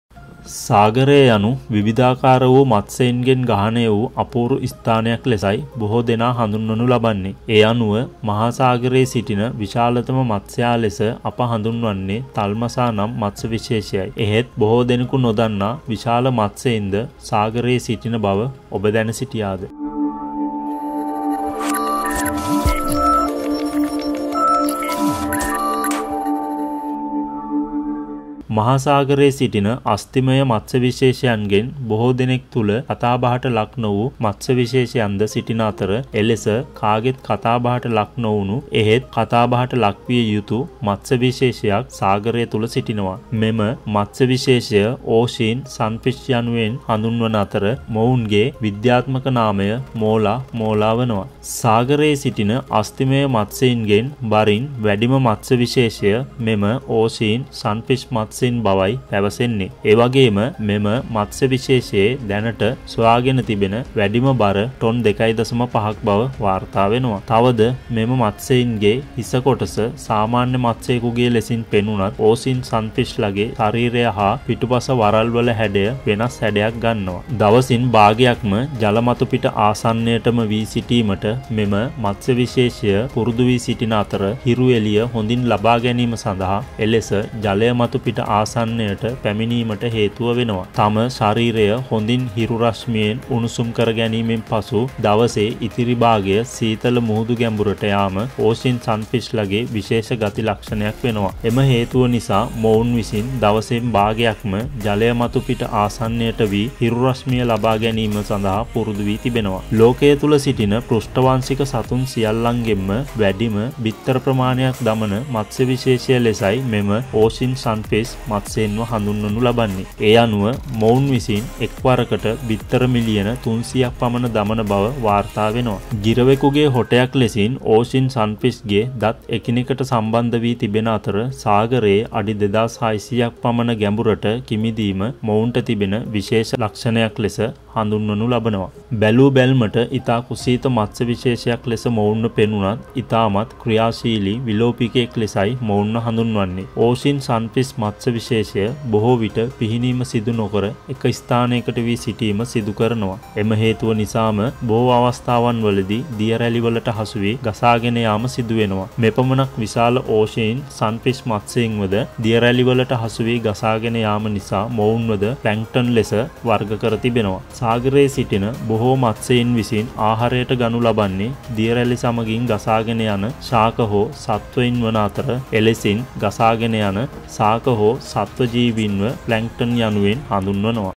මහ සාගරයේ සිටින විශාලතම අස්ථිමය මත්ස්‍යා....(Ocean Sunfish)
සාගරය යනු විවිධාකාර මත්ස්‍යයිනගෙන් ගහනය වු අපුරු ස්ථානයක් ලෙසයි බොහෝ දෙනා හඳුන්වන්නේ.ඒ අනුව මහ සාගරයේ සිටින විශාලතම මත්ස්‍යා ලෙස අප හඳුන්වන්නේ ත්මසා නම් මත්ස්‍ය විශේෂයයි.එහෙත් බොහෝ දෙනෙකු නොදන්නා විශාල මත්ස්‍යයින්ද මහ සාගරයේ සිටින බව ඔබ දැන සිටියාද. මේ එම මත්ස්‍යා ගැනයි. (Ocean Sunfish)

Viya Sidurin Lowa Desa
Lowa Desa

Video Clips Credited by
Pixabay & youtube

Photo Credited by
Google Image

සිදුරින් ලොව දෙස